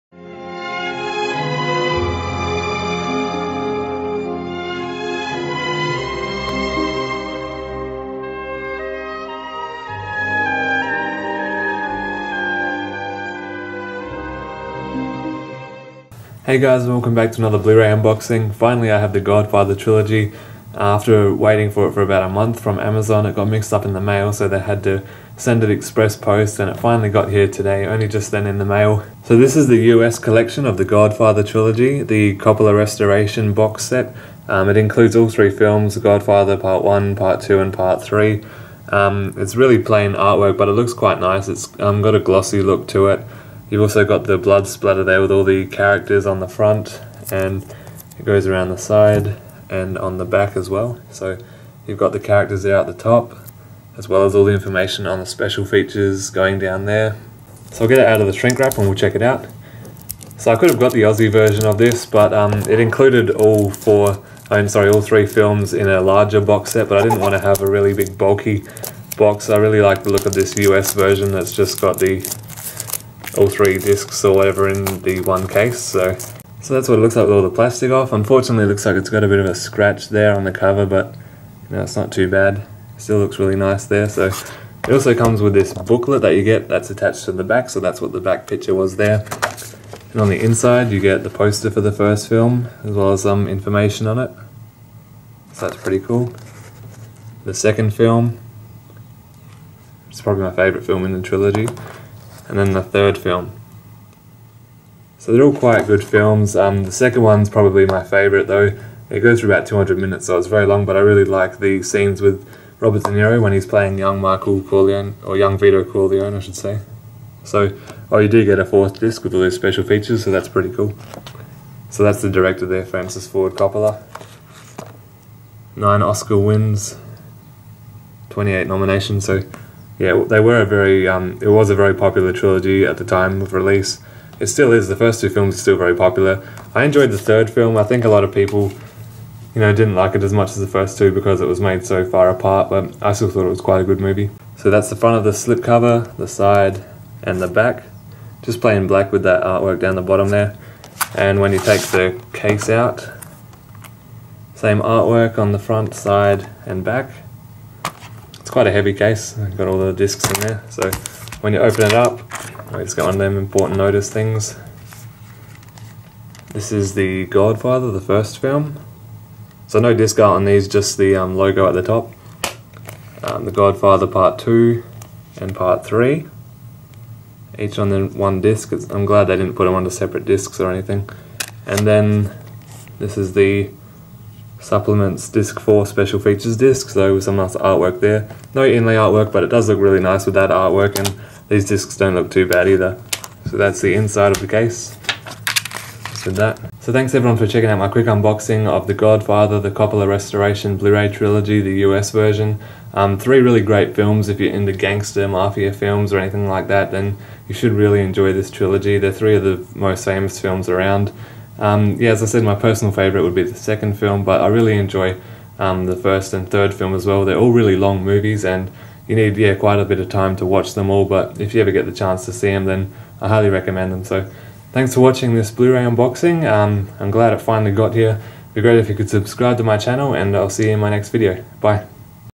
The Godfather Collection Blu-Ray Unboxing
I Unbox Francis Ford Coppola's trilogy 'The Godfather: The Coppola Restoration' on Blu-Ray